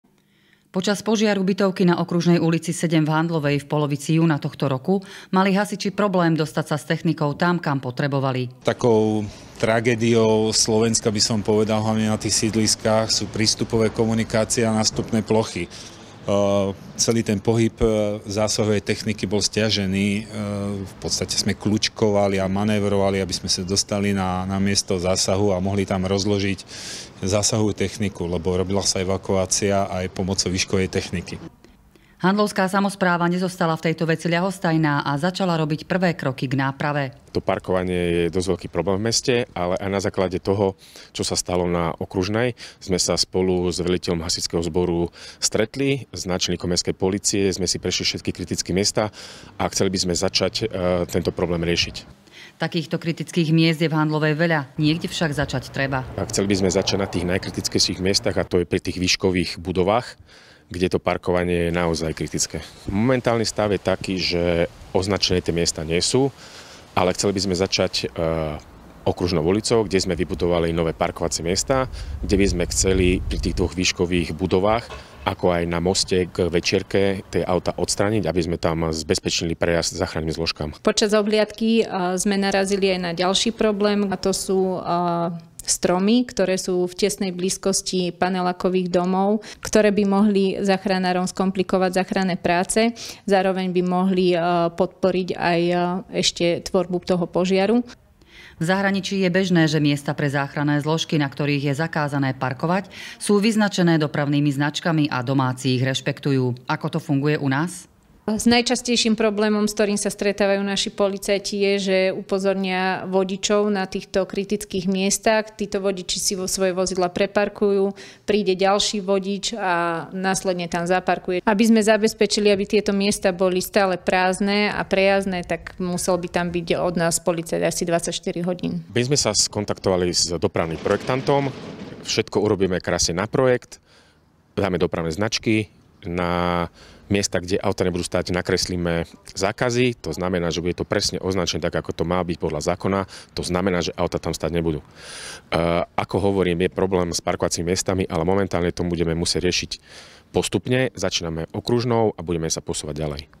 Voľná cesta pre záchranné zložky
Požiar bytovky na Okružnej ulici v Handlovej ukázal nedostatky, ktoré komplikovali prácu záchranným zložkám. Handlovská samospráva začala robiť prvé kroky k tomu, aby sa podobná situácia nezopakovala.